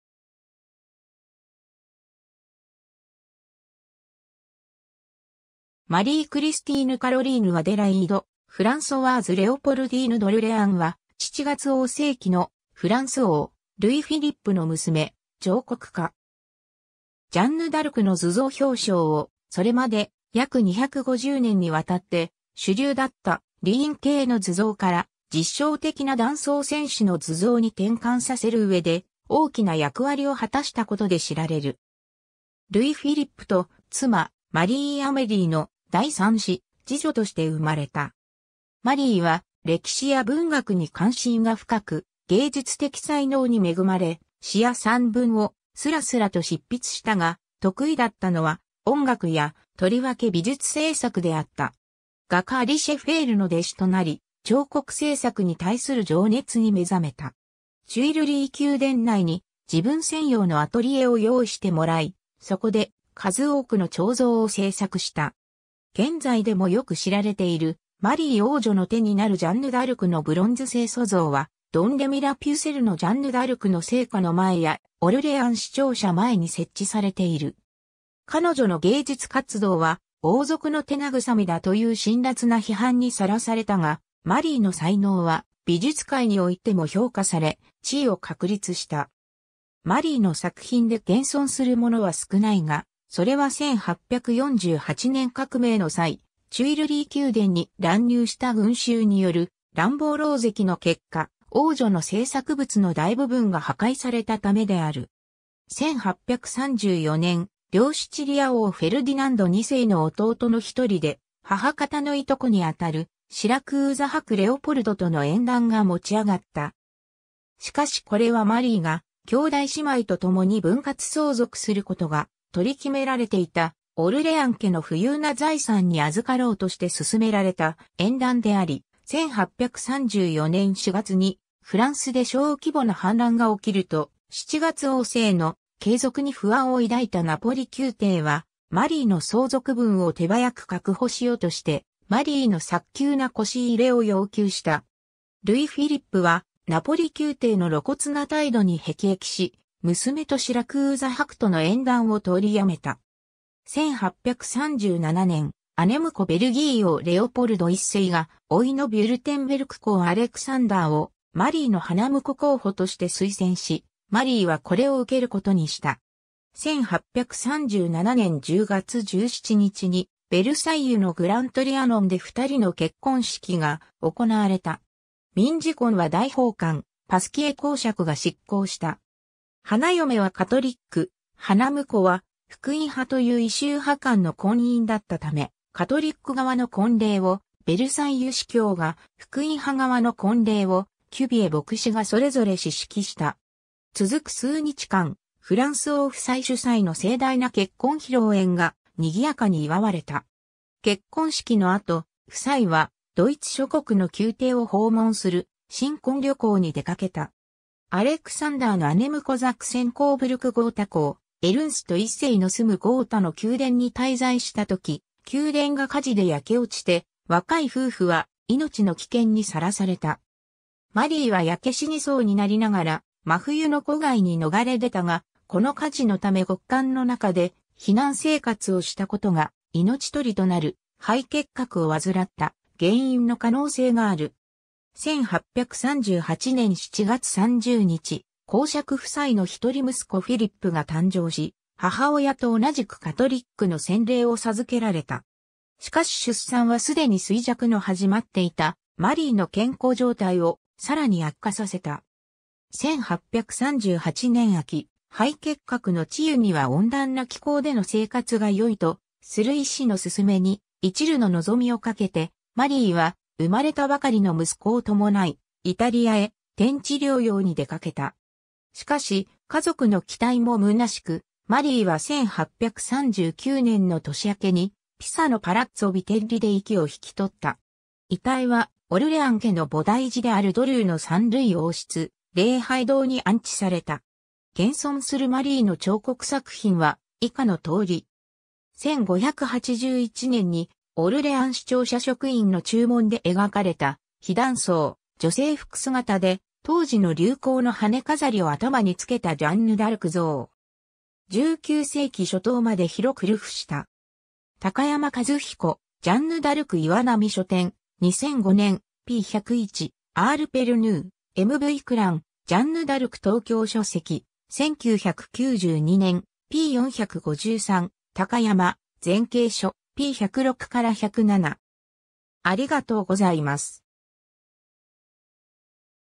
マリー・ドルレアン (1813-1839)
フランス王ルイ・フィリップの子女
マリー・クリスティーヌ・カロリーヌ・アデライード・フランソワーズ・レオポルディーヌ・ドルレアン（Marie Christine Caroline Adélaïde Françoise Léopoldine d’Orléans, 1813年4月12日 パレルモ - 1839年1月6日 ピサ）は、7月王政期のフランス王ルイ・フィリップの娘、彫刻家。
ジャンヌ・ダルクの図像表象を、それまで約250年にわたって主流だった「吏員系」の図像から、実証的な男装戦士の図像に転換させるうえで、大きな役割を果たしたことで知られる。
 ルイ・フィリップと妻マリー・アメリーの第3子、次女として生まれた。
 マリーは歴史や文学に関心が深く、芸術的才能に恵まれ、詩や散文をすらすらと執筆したが、得意だったのは音楽や、とりわけ美術制作であった。
画家アリ・シェフェールの弟子となり、彫刻制作に対する情熱に目覚めた。
テュイルリー宮殿内に自分専用のアトリエを用意してもらい、そこで数多くの彫像を制作した。
現在でもよく知られている、マリー王女の手になるジャンヌ・ダルクのブロンズ製塑像は、ドンレミ＝ラ＝ピュセルのジャンヌ・ダルクの生家の前や、オルレアン市庁舎前に設置されている。
彼女の芸術活動は「王族の手慰みだ」という辛辣な批判に晒されたが、マリーの才能は美術界においても評価され、地位を確立した。
マリーの作品で現存するものは少ないが、それは1848年革命の際、テュイルリー宮殿に乱入した群衆による乱暴狼藉の結果、王女の制作物の大部分が破壊されたためである。
 1834年、両シチリア王フェルディナンド2世の弟の1人で、母方の従兄に当たるシラクーザ伯レオポルドとの縁談が持ち上がった。
しかしこれはマリーが兄弟姉妹とともに分割相続することが取り決められていたオルレアン家の富裕な財産に与ろうとして進められた縁談であり、1834年4月にフランスで小規模な反乱が起きると、7月王政の継続に不安を抱いたナポリ宮廷は、マリーの相続分を手早く確保しようとしてマリーの早急な輿入れを要求した。
ルイ・フィリップはナポリ宮廷の露骨な態度に辟易し、娘とシラクーザ伯との縁談を取りやめた。
 1837年、姉婿ベルギー王レオポルド1世が甥のヴュルテンベルク公アレクサンダーをマリーの花婿候補として推薦し、マリーはこれを受けることにした。
1837年10月17日にヴェルサイユのグラン・トリアノンで2人の結婚式が行われた。
民事婚は大法官パスキエ公爵が執行した。
花嫁はカトリック、花婿は福音派という異宗派間の婚姻だったため、カトリック側の婚礼をヴェルサイユ司教が、福音派側の婚礼をキュヴィエ牧師がそれぞれ司式した。
続く数日間、フランス王夫妻主催の盛大な結婚披露宴がにぎやかに祝われた。
 結婚式の後、夫妻はドイツ諸国の宮廷を訪問する新婚旅行に出かけた。
アレクサンダーの姉婿ザクセン＝コーブルク＝ゴータ公エルンスト1世の住むゴータの宮殿に滞在したとき、宮殿が火事で焼け落ちて、若い夫婦は命の危険にさらされた。
マリーは焼け死にそうになりながら真冬の戸外に逃れ出たが、この火事のため極寒の中で避難生活をしたことが、命取りとなる肺結核を患った原因の可能性がある。
1838年7月30日、公爵夫妻の一人息子フィリップが誕生し、母親と同じくカトリックの洗礼を授けられた。
しかし出産はすでに衰弱の始まっていたマリーの健康状態をさらに悪化させた。
 1838年秋、肺結核の治癒には温暖な気候での生活が良いとする医師の勧めに一縷の望みをかけて、マリーは生まれたばかりの息子を伴い、イタリアへ転地療養に出かけた。
しかし家族の期待も空しく、マリーは1839年の年明けにピサのパラッツォ・ヴィテッリ（Palazzo Vitelli）で息を引き取った。
遺体はオルレアン家の菩提寺であるドルーのサン＝ルイ王室礼拝堂に安置された。
 現存するマリーの彫刻作品は以下の通り。
 ^ 1581年にオルレアン市庁舎職員（吏員）の注文で描かれた、非男装・女性服姿で、当時の流行の羽飾りを頭に付けたジャンヌ・ダルク像。
19世紀初頭まで広く流布した。
高山一彦『ジャンヌ・ダルク』岩波書店（岩波新書）、2005年、P101 ^...